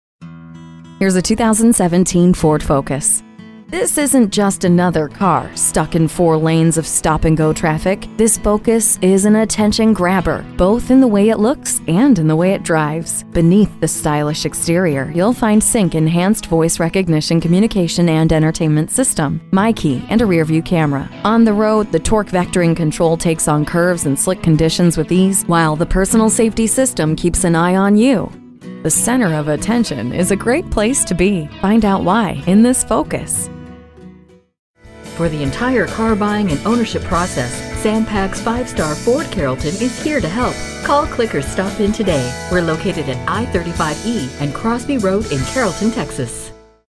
New 2017 Ford Focus Dallas Ft Worth DFW, TX
Year: 2017
Make: Ford
Model: Focus
Trim: ST
Engine: 4 Cyl. 2.0 L
Transmission: Manual
Color: Kona Blue
Interior: Blk Clth Sts
Stock #: HL310286
VIN: 1FADP3L92HL310286
2017 Ford Focus ST 30/22 Highway/City MPG

Address:
1635 Interstate 35